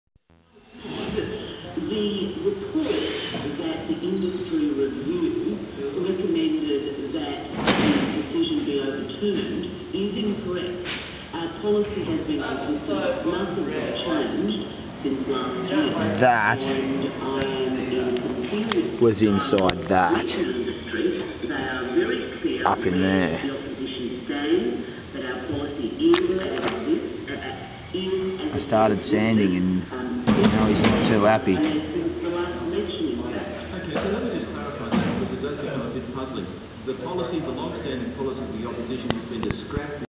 Mini bat found inside air sander
nice start to my day at 6 am